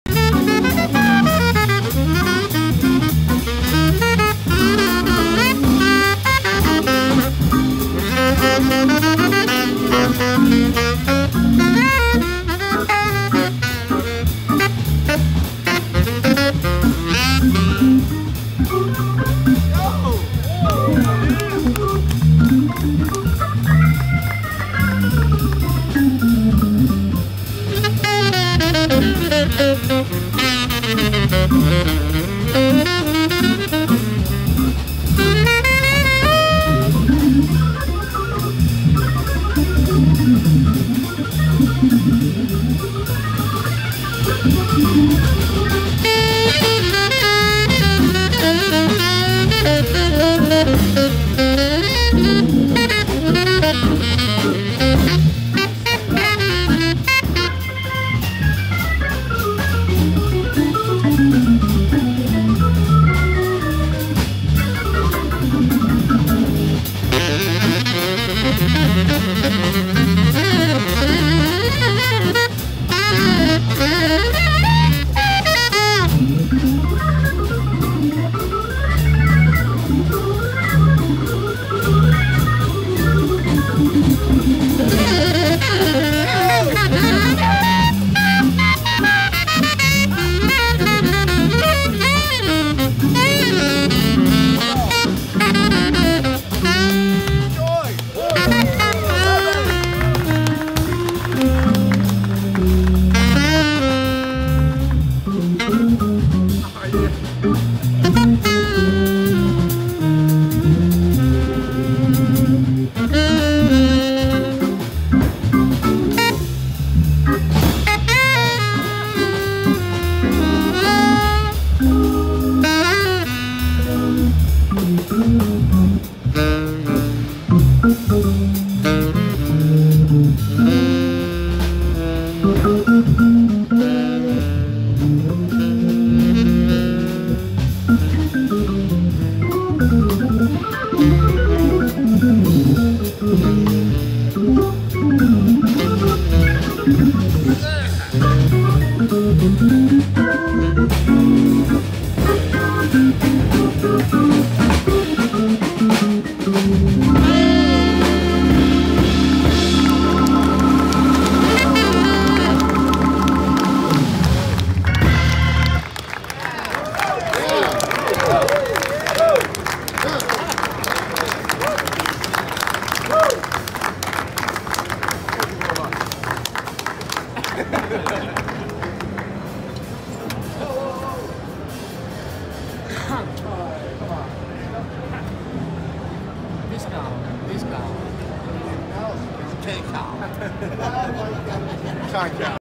Joey DeFrancesco Performance at Viscount Booth At NAMM January 18, 2020 (Part 2)
Joey DeFrancesco performance at the Viscount Booth at Anaheim NAMM on January 18, 2020 around 11 am.  This is part 2. Troy Roberts on saxophone.